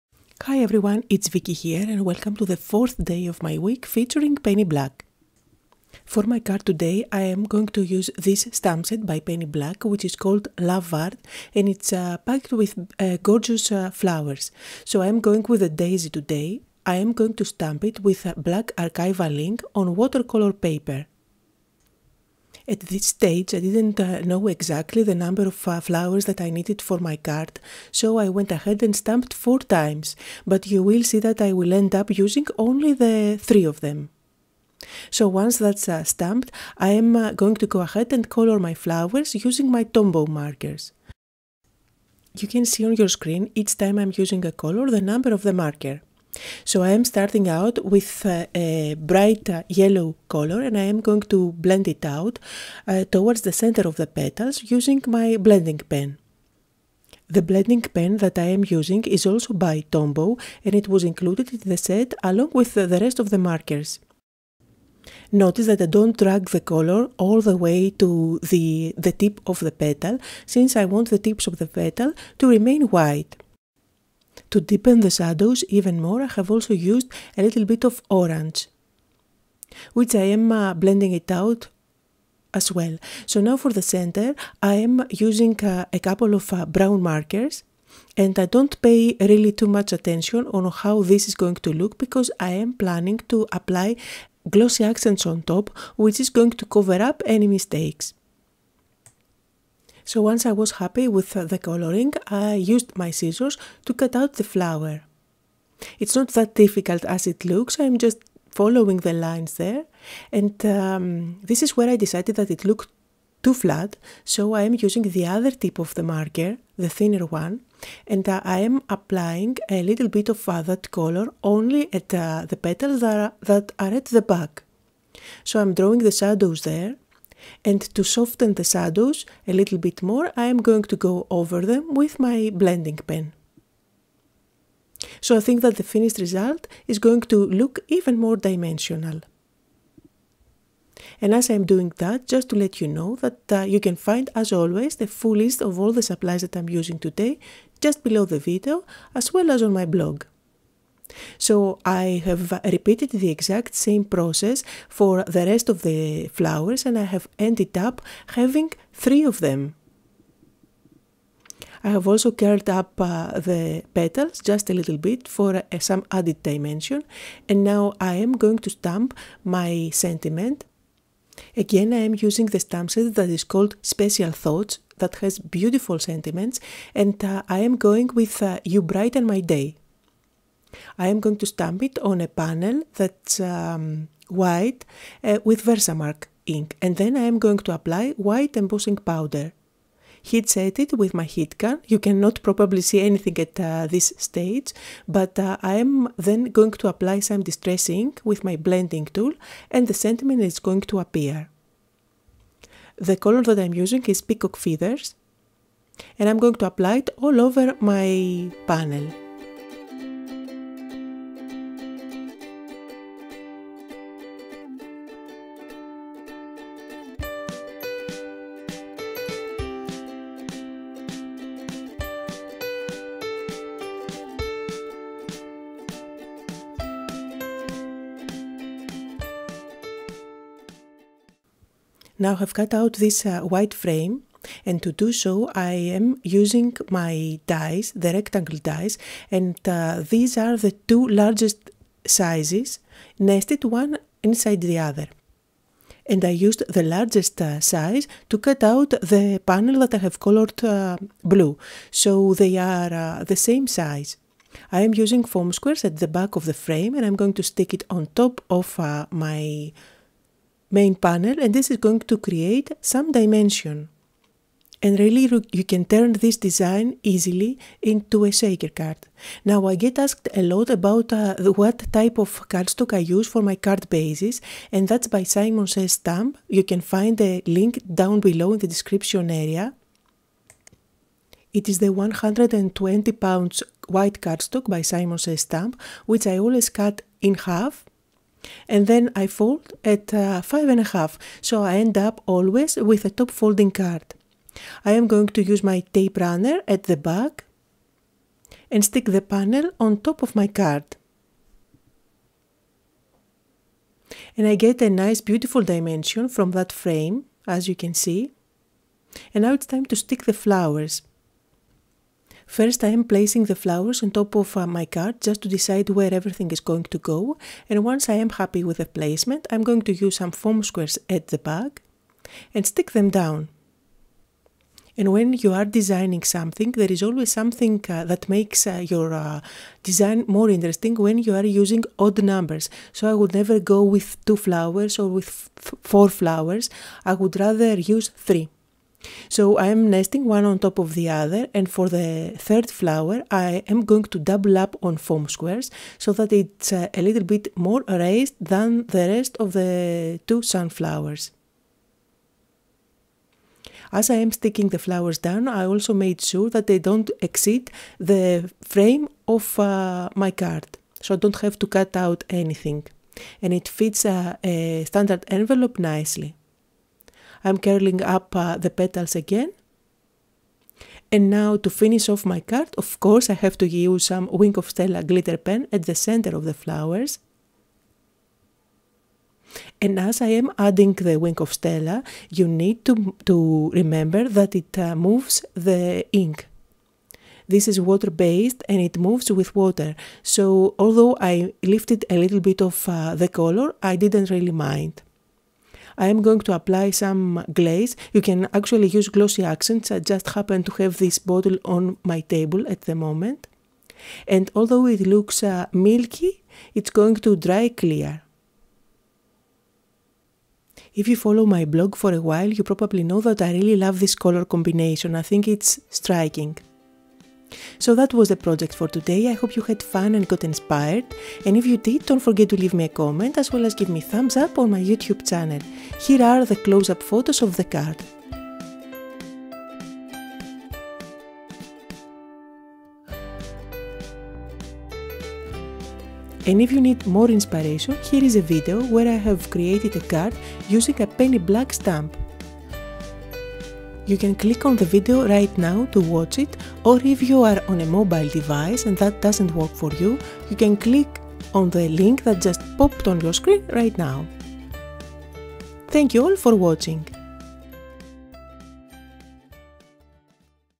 Flower card | Featuring Penny Black - DAY 4
Step by step video on creating a card using stamps by Penny Black and coloring with Tombow Dual brush pens.